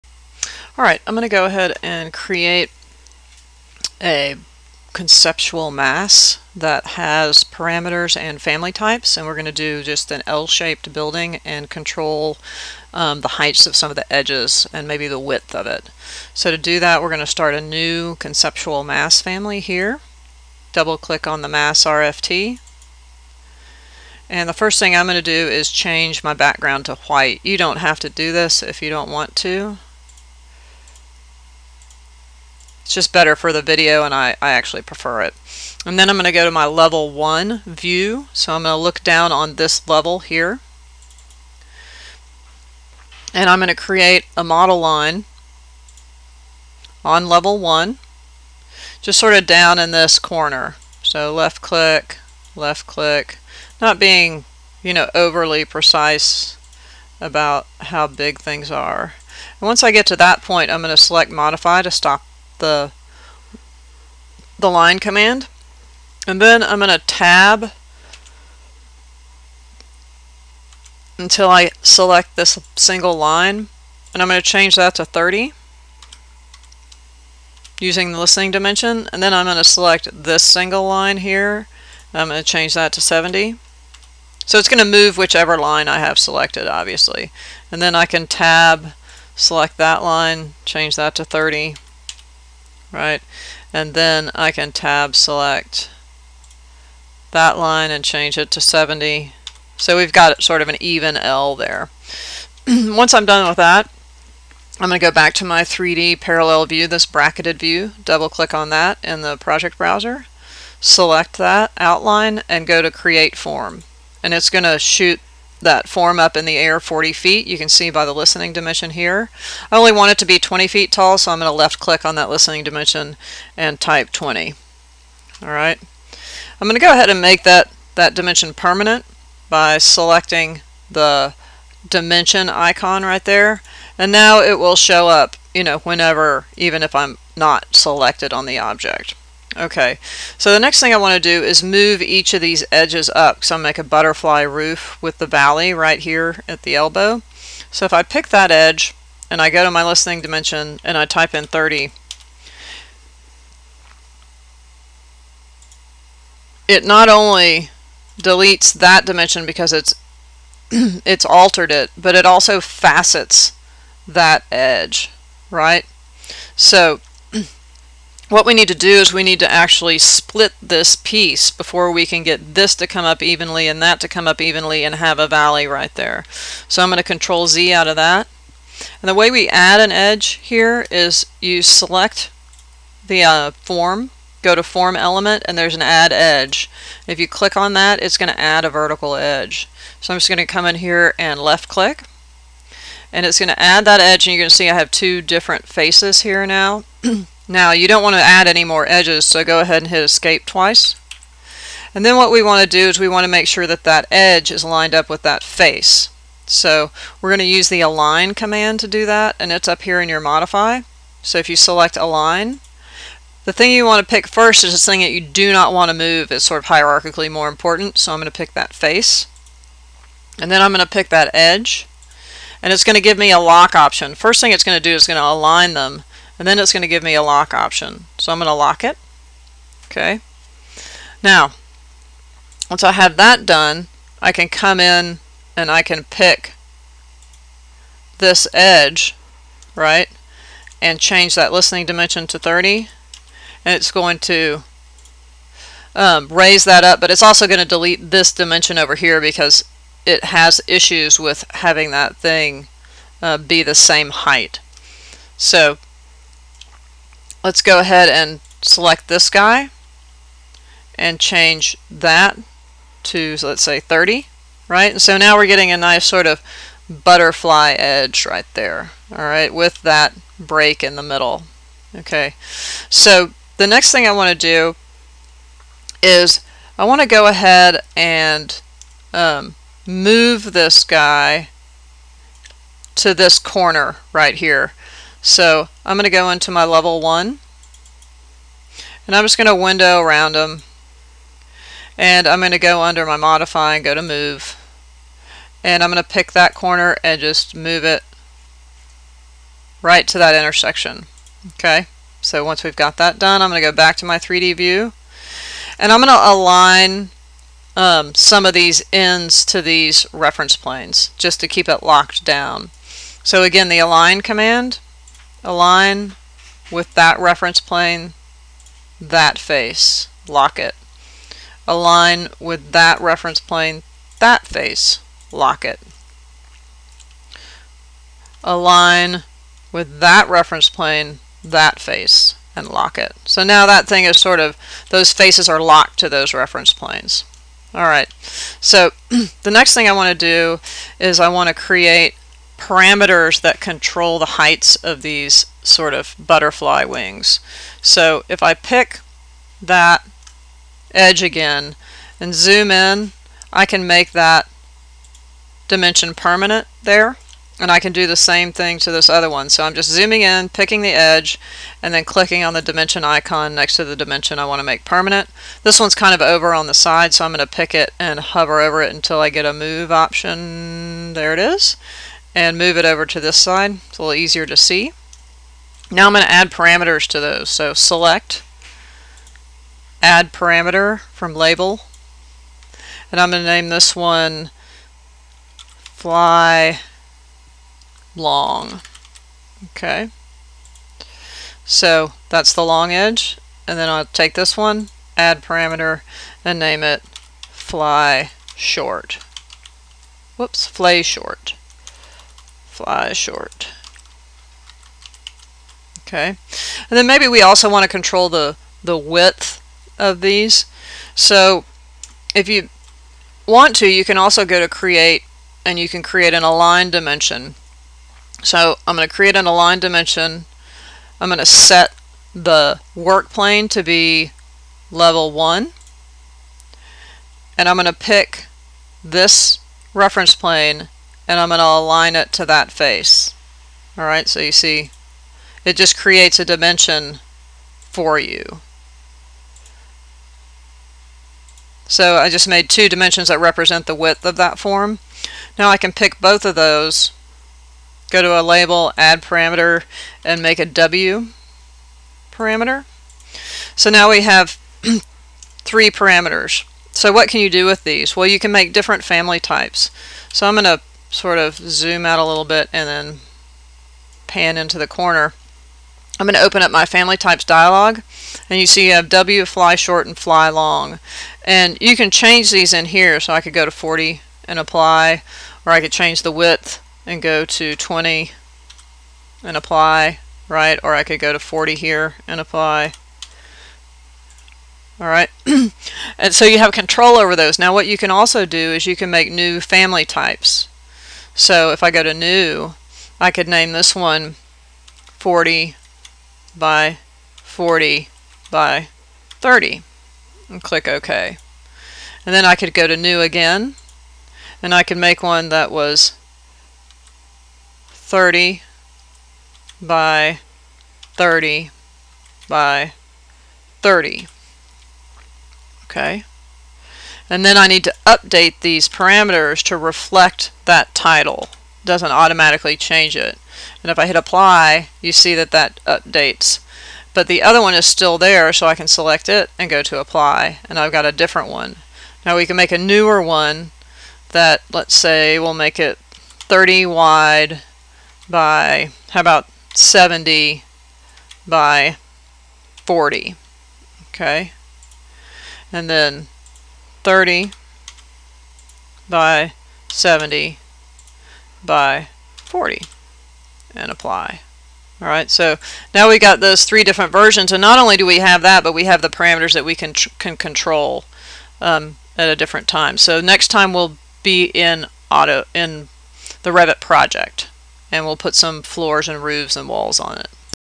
REVIT CONCEPTUAL MASSING 03 INRTO TO PARAMETERS AND FAMILY TYPES
Intro to schematic Conceptual Massing with simple parameters using model lines, parameters, and family types.